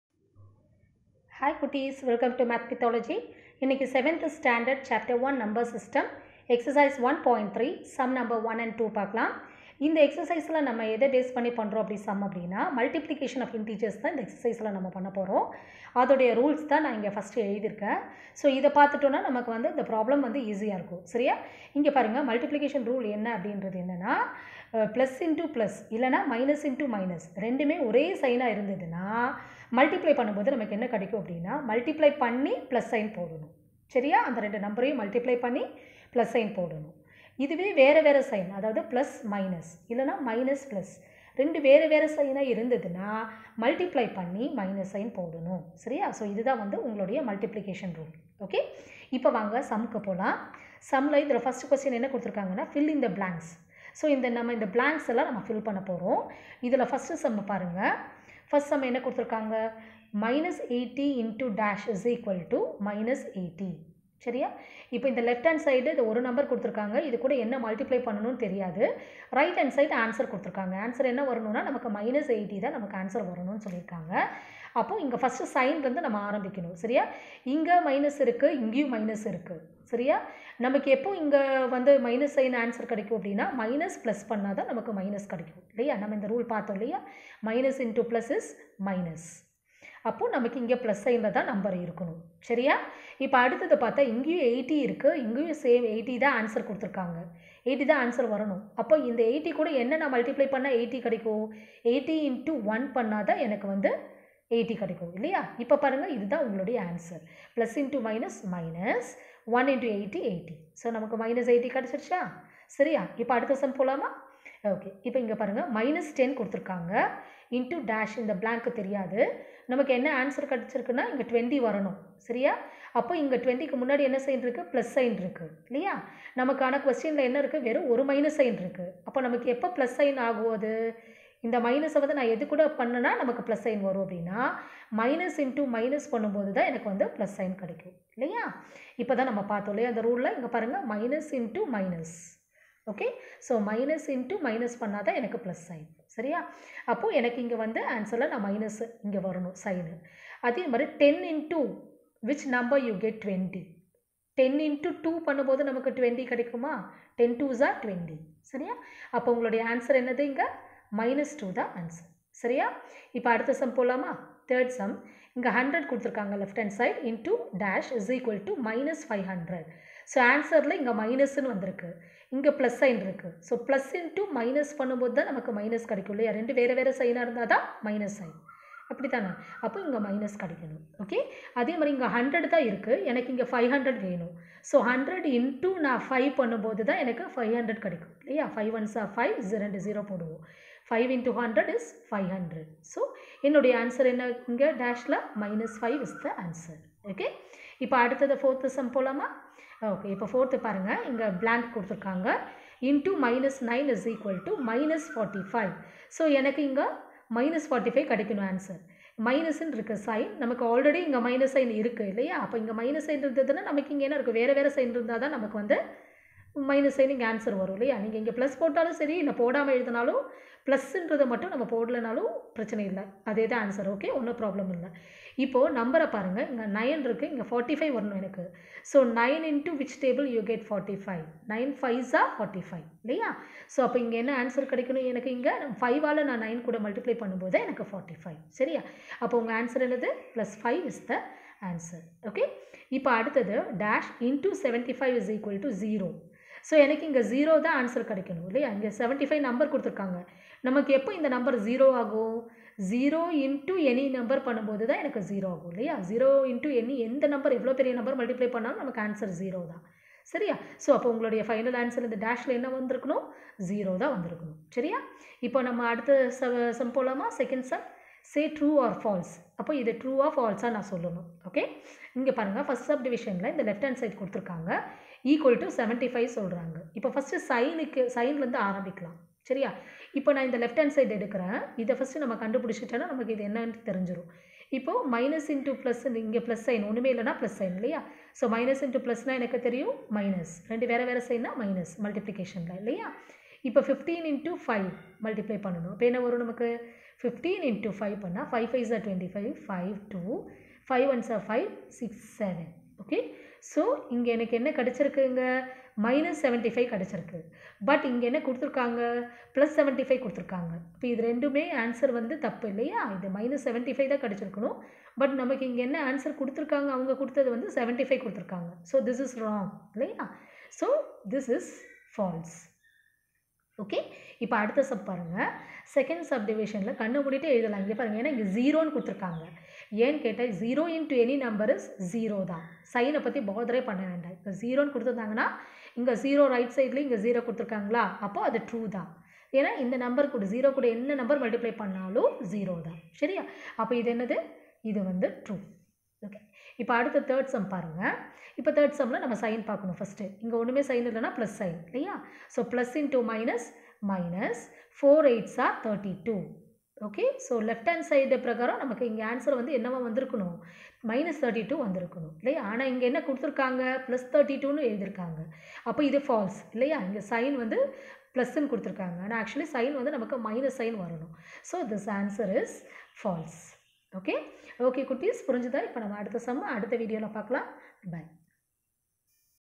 TN Samacheer Class 7 Maths New Syllabus|Chapter-1|Term-1|Number system| Ex : 1.3|Sum No: 1 & 2.
In this video you learn about TN Samacheer class 7 Maths New Syllabus Chapter-1 Number System Ex: 1.3 Sum: 1& 2 solutions in Tamil.

ex: 1.3 Sum: 1
ex: 1.3 Sum: 2